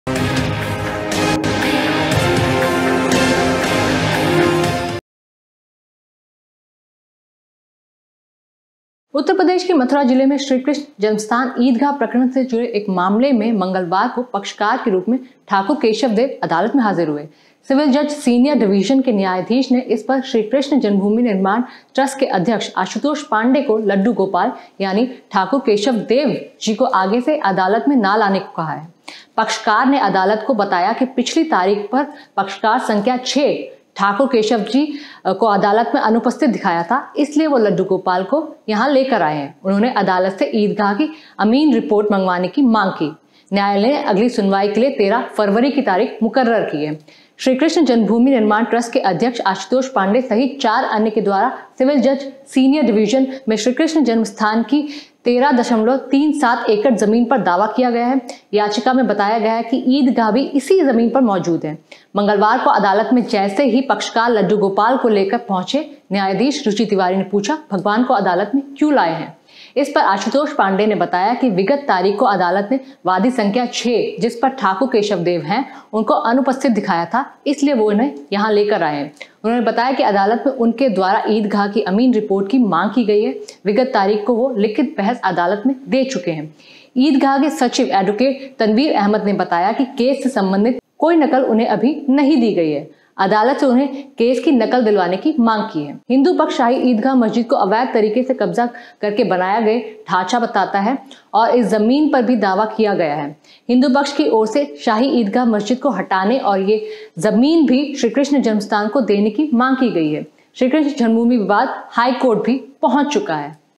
Mathura: अदालत में हाजिर हुए लड्डू गोपाल, जज बोली- मान लिया है भगवान, अगली डेट पर इन्हें न लाएं आप
अदालत में हाजिर हुए लड्डू गोपाल, जज बोली- हमने मान लिया है भगवान, अगली डेट पर इन्हें न लाएं आप